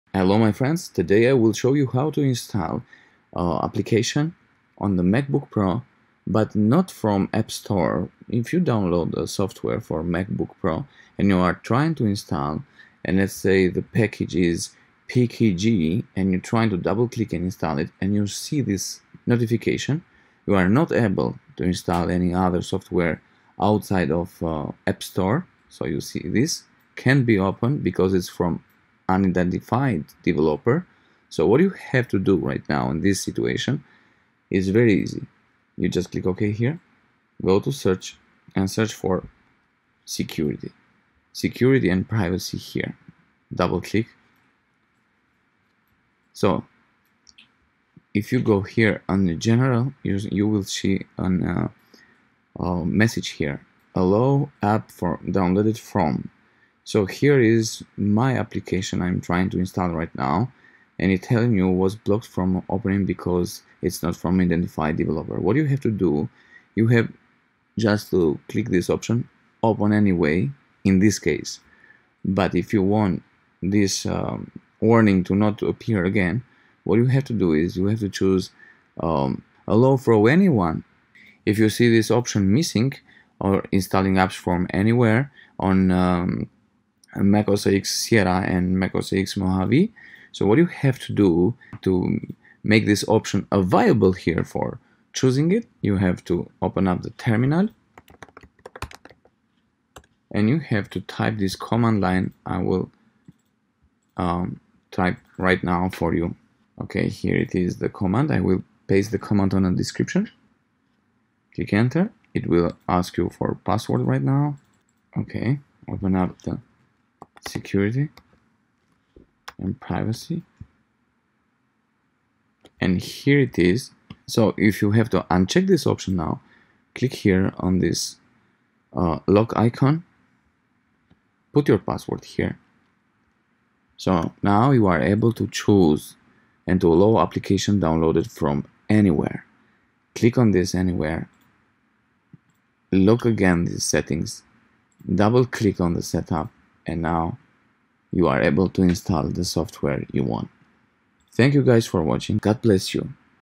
[Fixed]Can’t be opened because it is from an unidentified developer - Allow Apps from Anywhere macOS
MacOS is a sophisticated operating system with a high level of security. By default, it does not allow you to install applications from unidentified developers. However,
1. Open Up “Security & Privacy” and the “General” tab
You will now see the “Anywhere” option under ‘Allow apps downloaded from:
if you don't see it than :
2. Open the Terminal app from the /Applications/Utilities/ folder and then enter the following command syntax:
sudo spctl --master-disable
3. Open Up again  “Security & Privacy” and the “General” tab

4.You can see a little lock icon with in yellow on the bottom left of the window , click to unlock , put your password, than chose the option
“Anywhere”